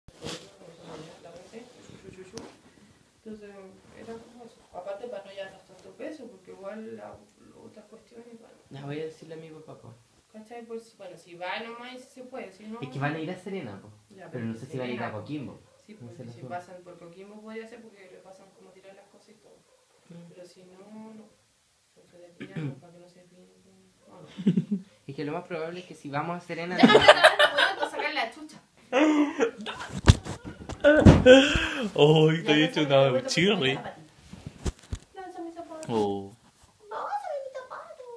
una loba arrebatada
-En nuestras vacaciones, me pongo a grabar a amina y ella se enoja y e tira un zapato